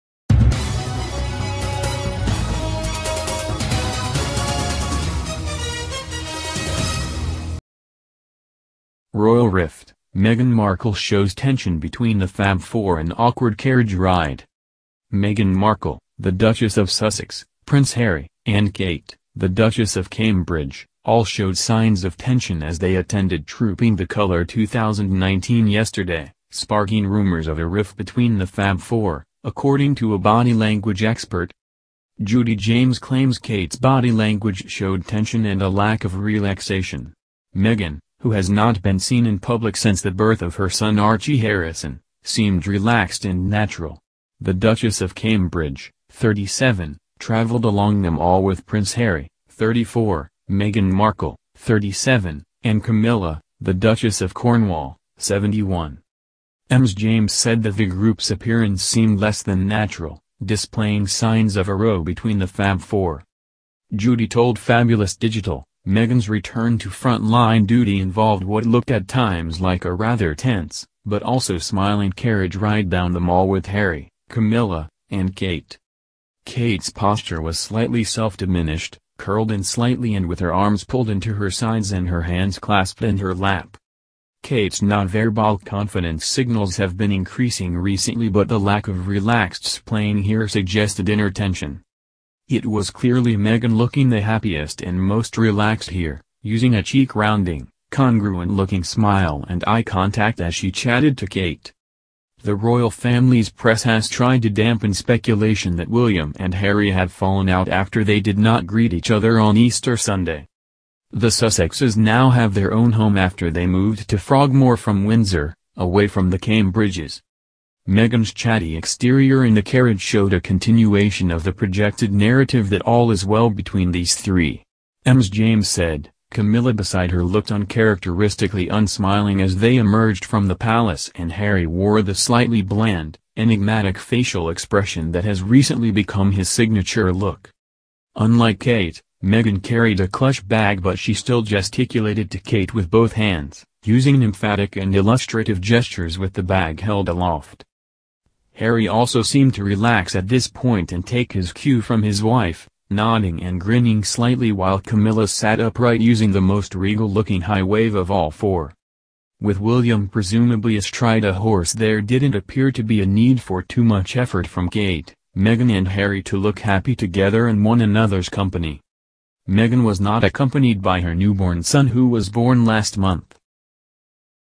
Royal Rift: Meghan Markle exhibits a "tension" with Duchess Kate, on a trip with the AWKWARD
Royal Rift: Meghan Markle exhibits a "tension" with the Duchess Kate, on a trip with the AWKWARD